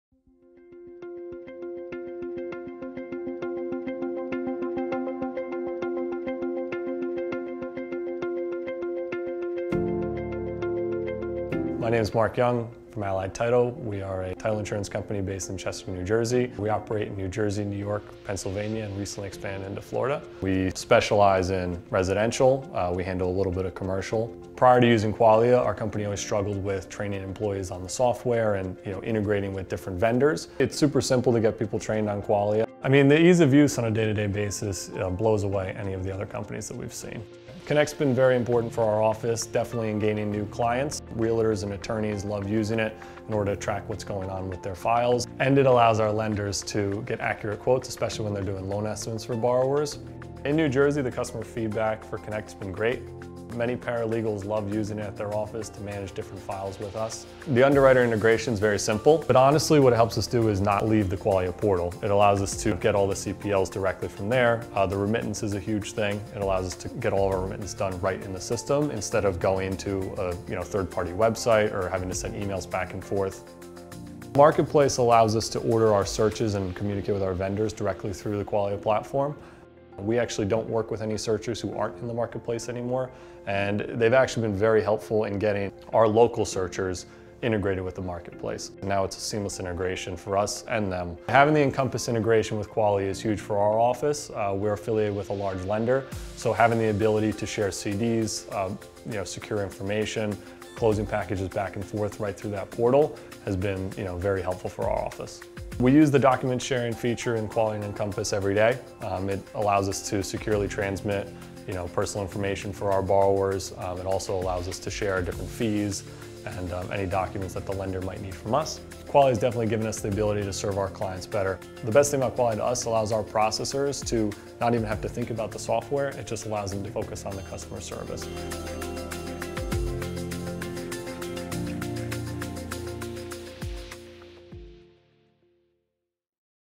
How Allied Title Provides a Superior Closing Experience With Qualia
Allied Title shares how the Qualia Platform has empowered them to provide a superior closing experience. From seamless employee onboarding to in-app efficiency, Qualia provides Allied Title with an ideal business solution.

Learn more about how Allied Title uses Qualia to close both residential and commercial real estate transactions